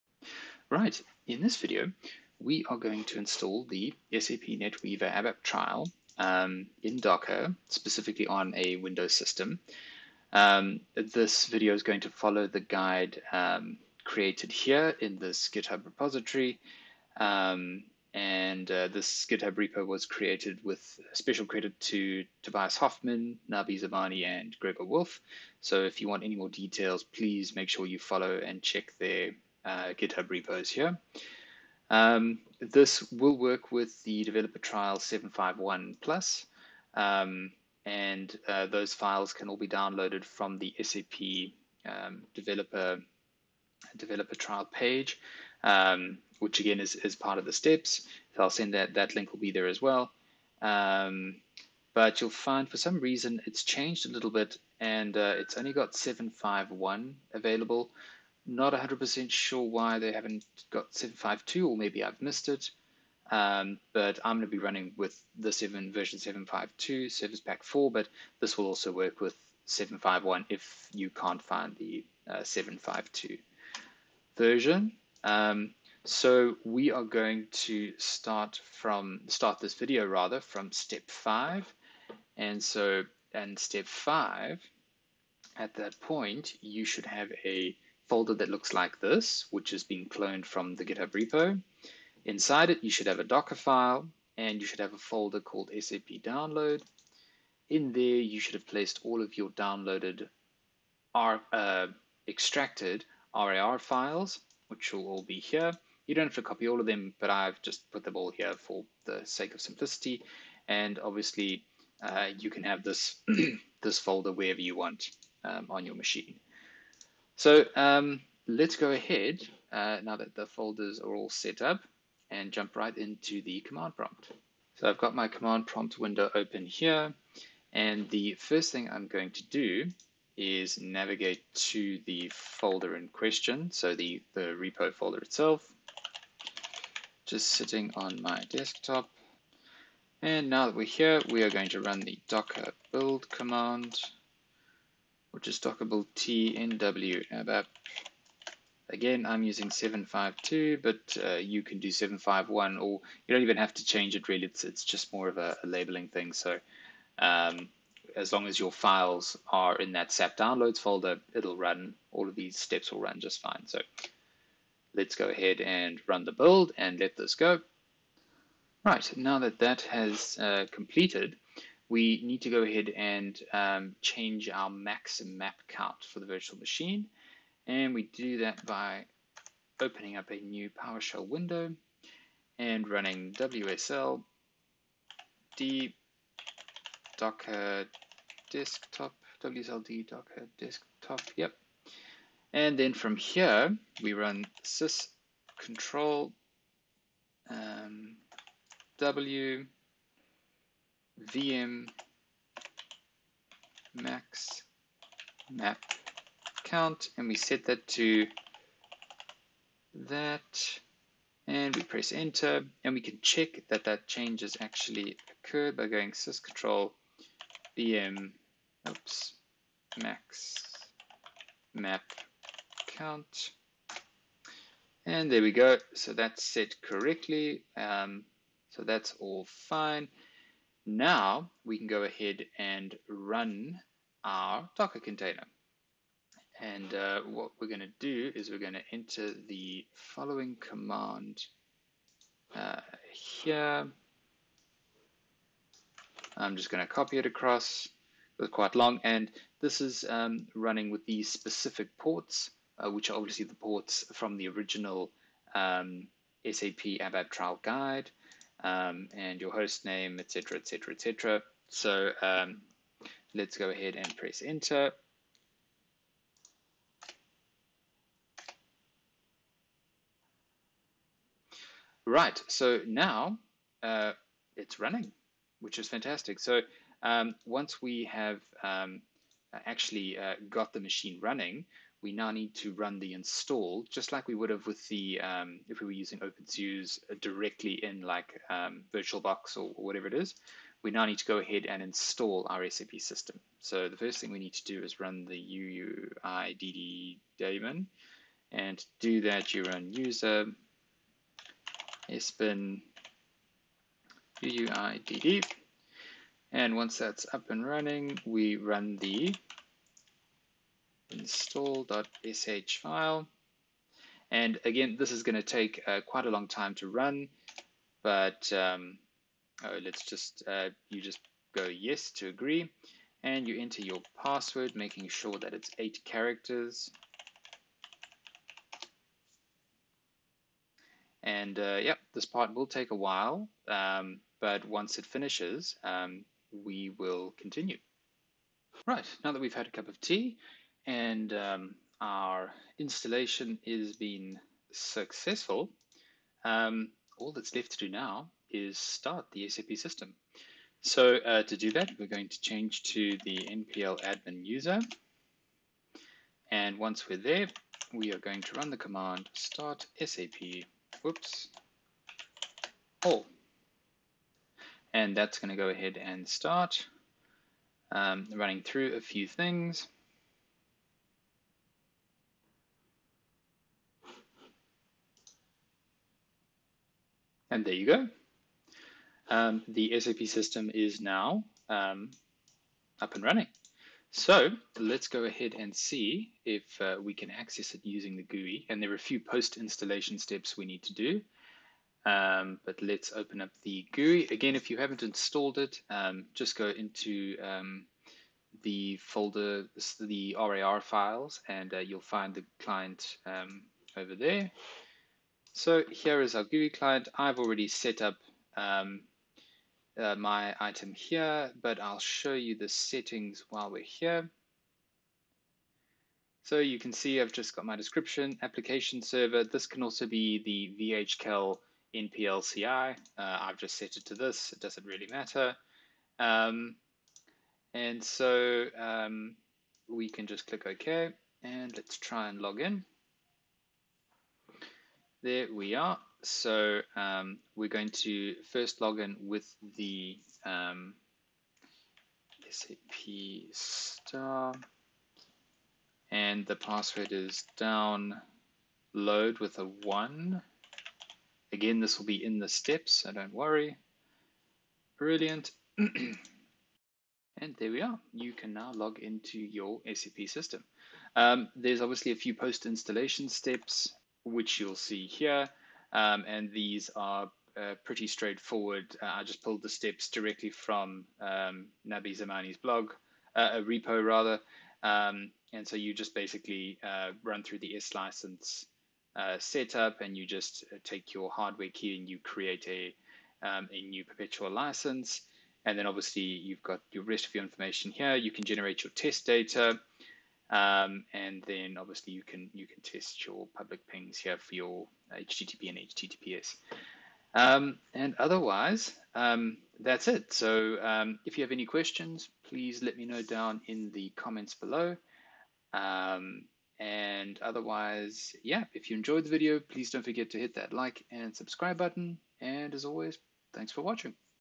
SAP ABAP Trial in Docker | Windows Setup Guide [Tutorial]
Learn how you can run your local SAP NetWeaver ABAP Trial instance in Docker on Windows using this setup guide

Tested on version 7.51 SP02 and above.